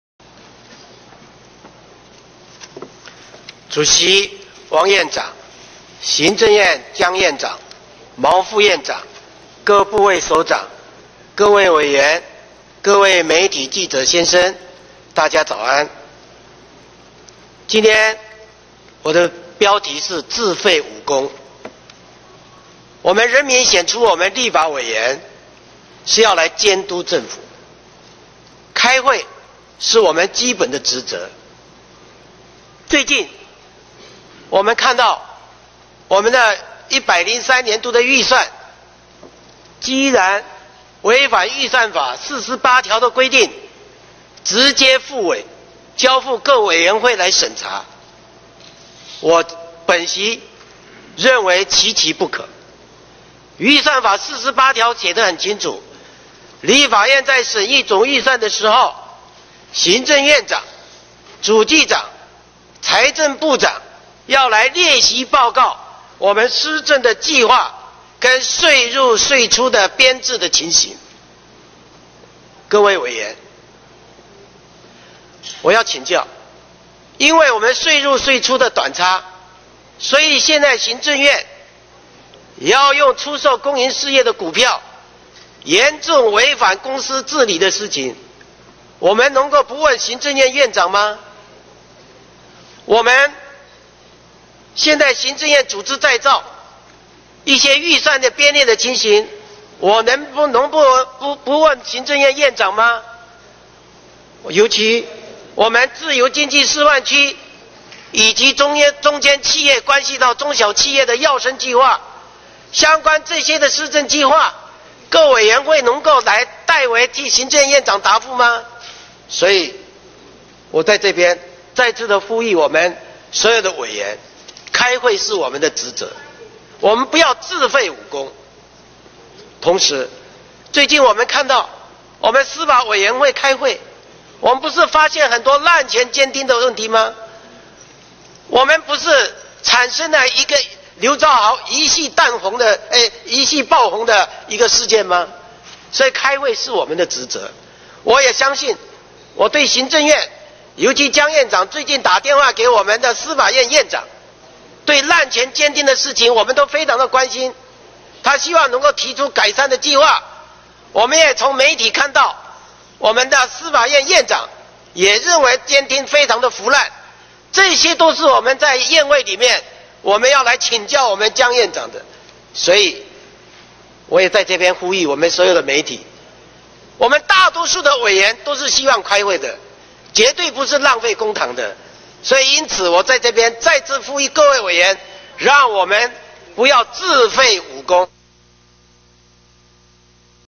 2013-10-04 廖正井 發言片段, 第8屆第4會期第4次會議
會議簡介: 第8屆第4會期第4次會議一、4日上午9時至10時為國是論壇時間。二、行政院院長施政報告並備質詢。三、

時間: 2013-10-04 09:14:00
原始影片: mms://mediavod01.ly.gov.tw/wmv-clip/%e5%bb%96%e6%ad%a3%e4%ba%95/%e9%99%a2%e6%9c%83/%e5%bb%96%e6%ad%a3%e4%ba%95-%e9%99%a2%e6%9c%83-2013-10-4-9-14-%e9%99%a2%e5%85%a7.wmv